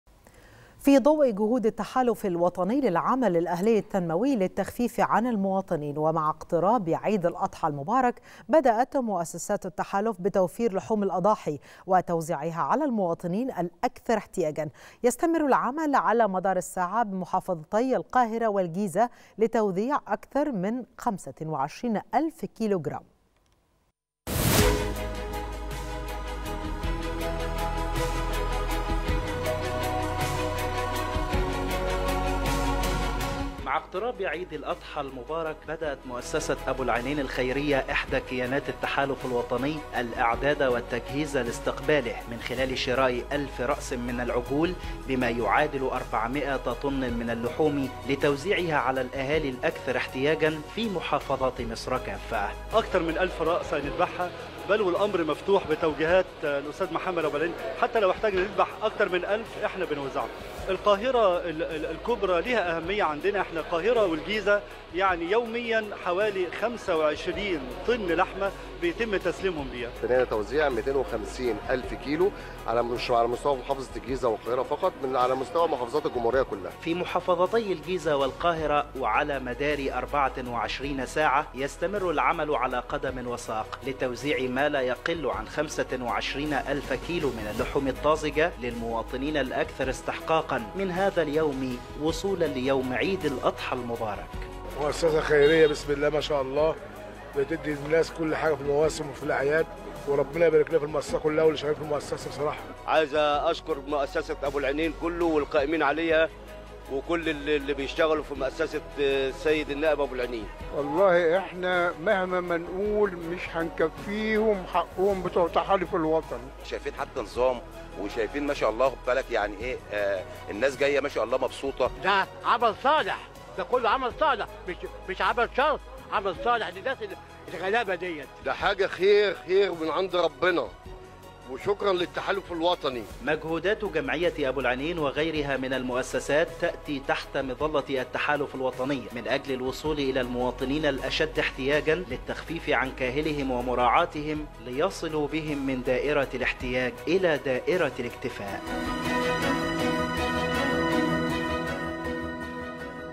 السادسة | التحالف الوطني للعمل الأهلي يبدأ توزيع اللحوم على الأسر الأولى بالرعاية بالقاهرة والجيزة
نشرة السادسة | التحالف الوطني للعمل الأهلي يبدأ توزيع اللحوم على الأسر الأولى بالرعاية بالقاهرة والجيزة